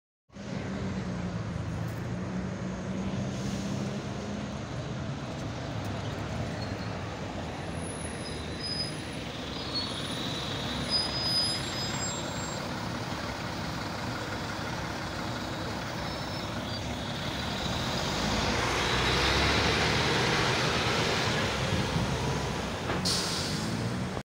VOLVO 7700A FJX-199 BKV BKK 17 VILLAMOS POTLÓ BUSZON SZÉL KÁLMÁN TÉR INDUL TOVÁBB
VISZONYLAT 17.BUSZ.
TELEPHELY KELENFŐLDI AUTÓBUSZGARÁZS.
ÜZEMETETŐ BKV
RENDSZÁM  FJX-199